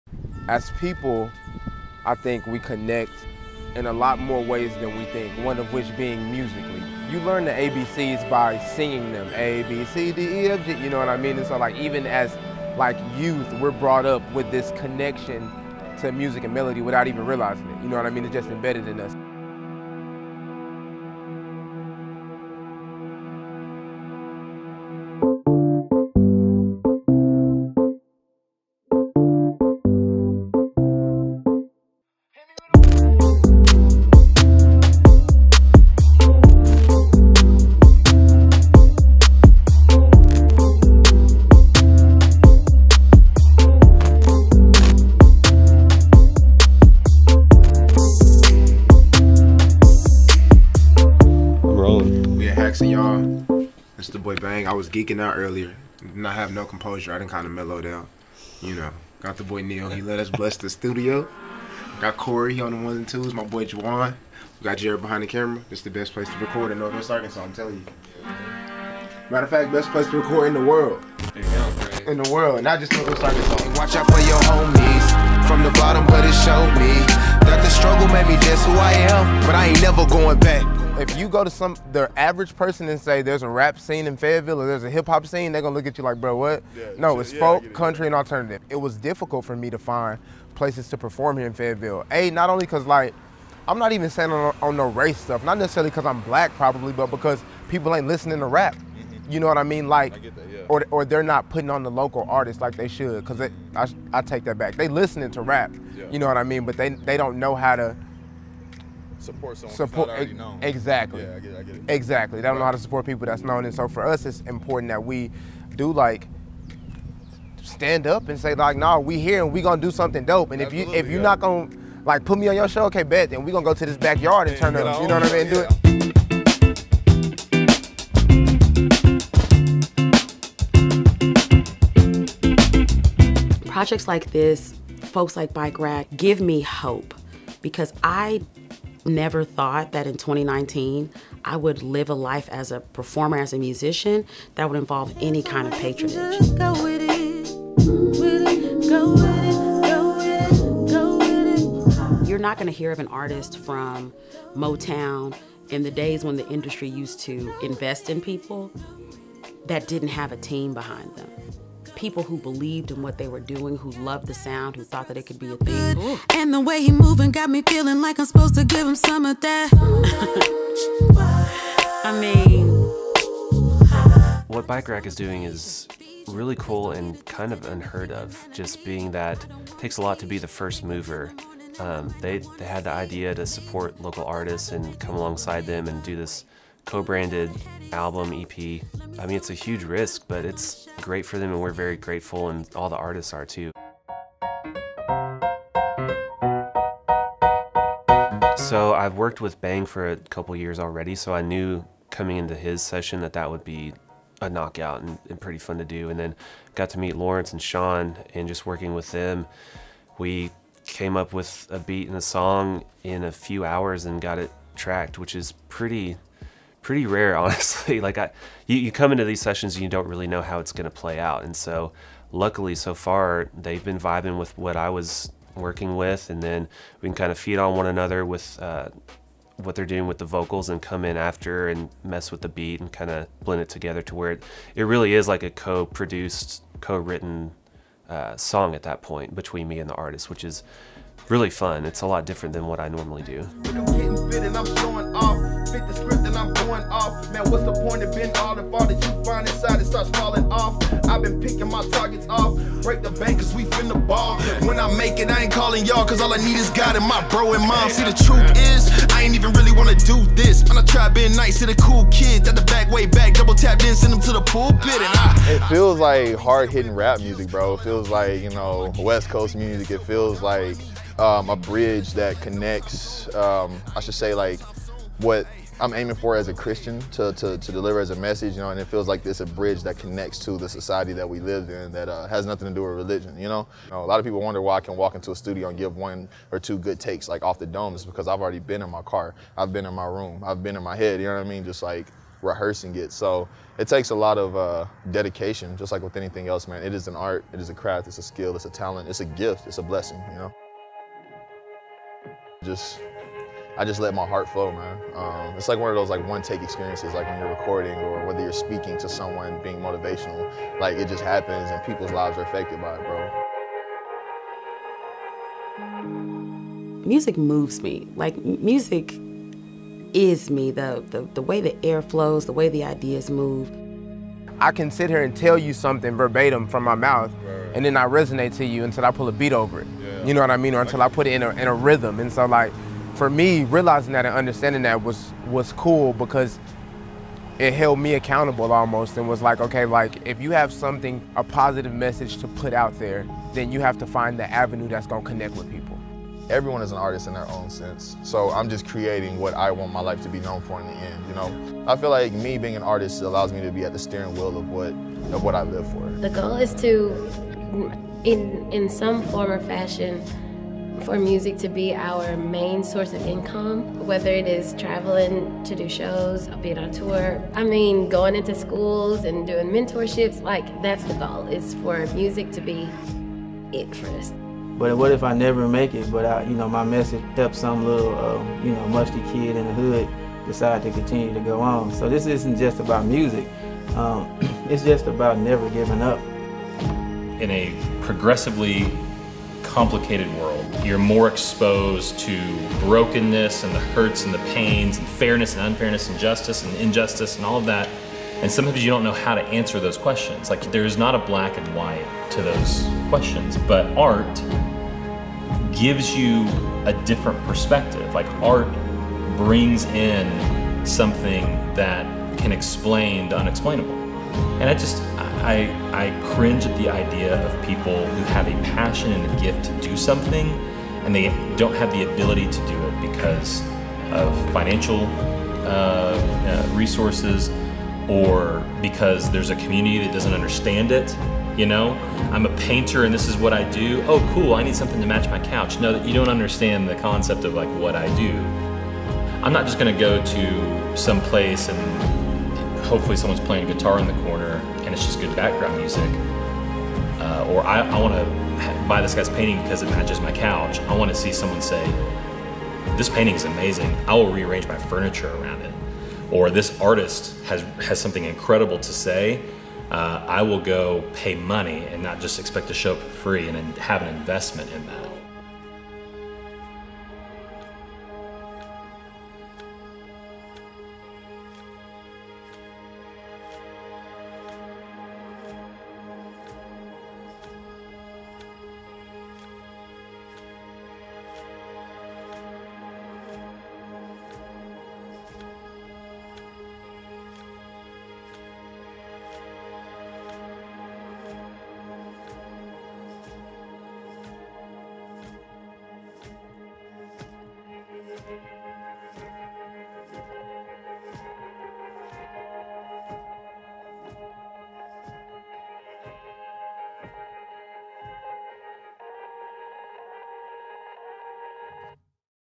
Bike Rack Records Vol. 2
Bike Rack Brewing Co in collaboration with Jerad Sears (City Sessions), Haxton Road Studios, Visit Bentonville, The House of Songs, First National Bank, and Ozark Collective (Jared Sluyter) partnered together to capture artists from NW Arkansas who are making incredible music from an underground hip-hop scene to country to everything in between. This documentary celebrates our diverse music and artists that live here.